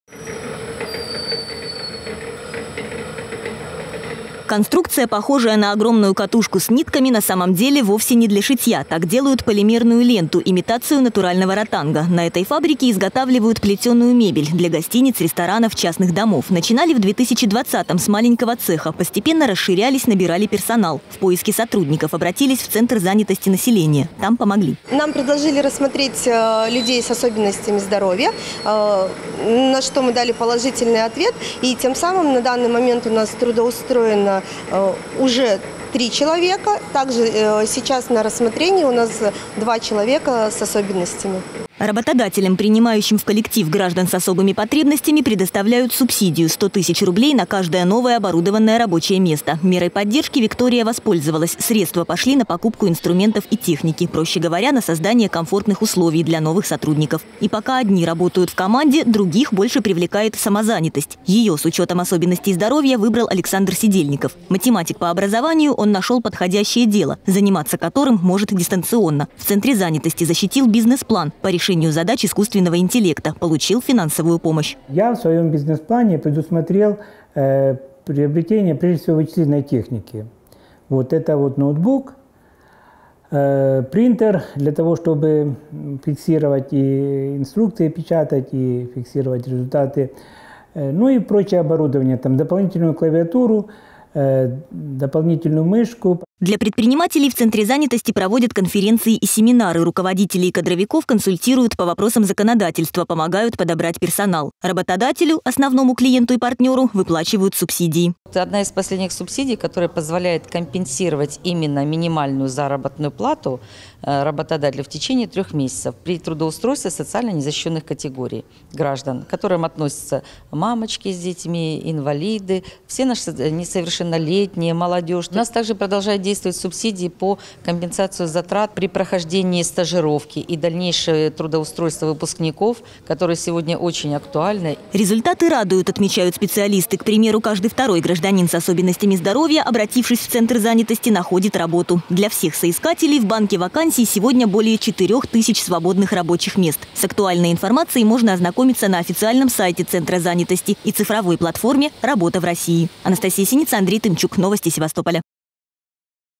В Центре занятости помогают трудоустроиться людям с особенностями здоровья
Оборудовать новые рабочие места с учётом всех потребностей работников с особенностями здоровья, создавать комфортные условия для труда и отдыха персонала предпринимателям помогают в Центре занятости. На какие ещё меры поддержки могут рассчитывать севастопольские работодатели?